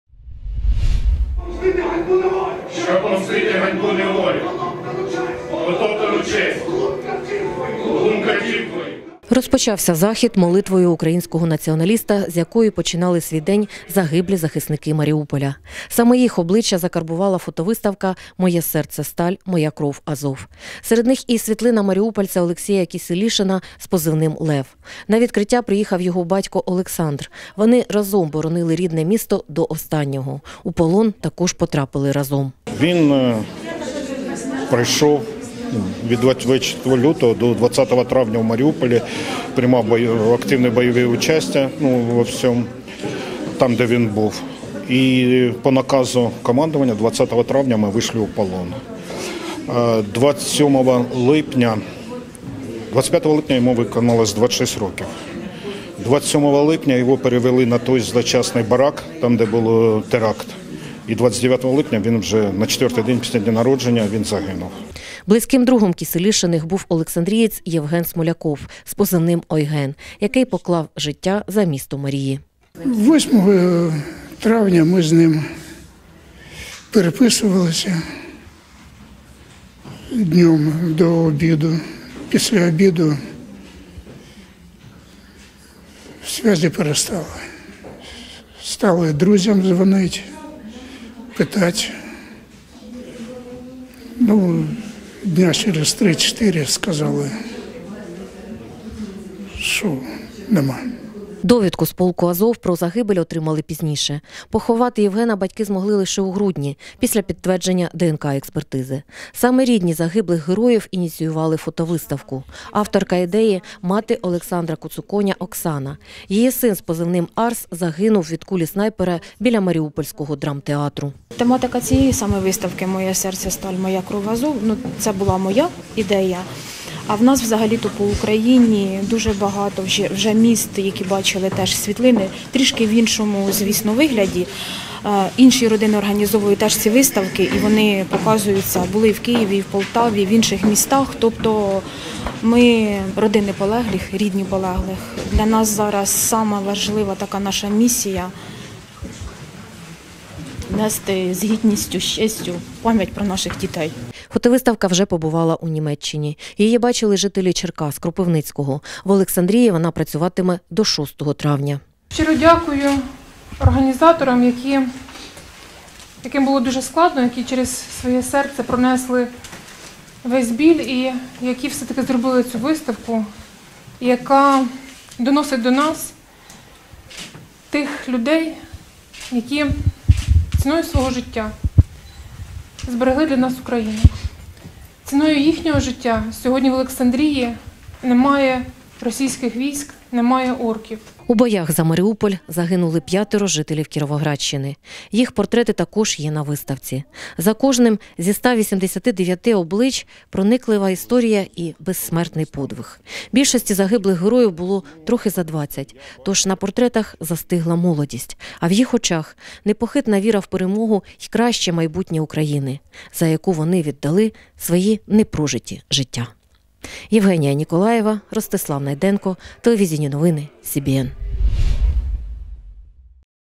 Виставка, присвячена загиблим оборонцям Маріуполя, відкрилася в Олександрії | CBN
«Моє серце – сталь, моя кров – Азов». Пересувна виставка під такою назвою відкрилася в Олександрії. Присвячена вона загиблим оборонцям Маріуполя. Протягом місяця жителі та гості міста мають змогу зазирнути в очі загиблим героям, які своїми життями зупинили ворожу навалу навесні минулого року і змінили хід війни.